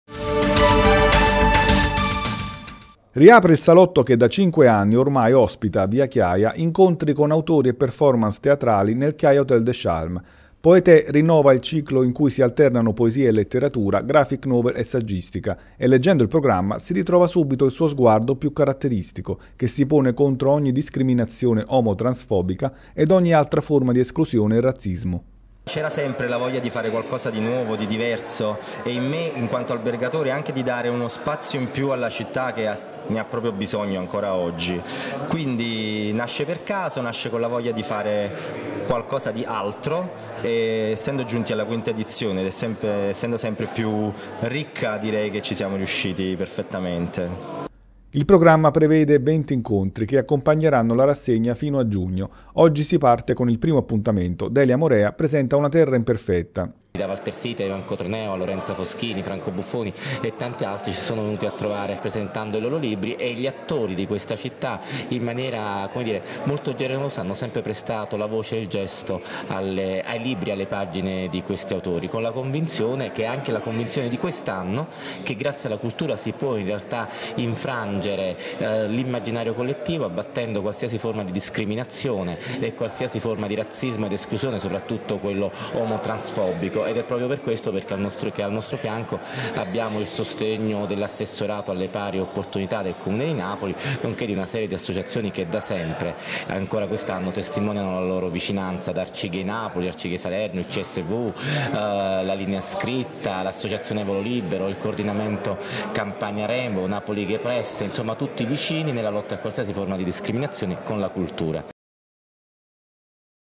Poetè: l'arte contro le discriminazioni
Parte oggi la quinta edizione della rassegna: in programma incontri con autori e performance teatrali fino a giugno.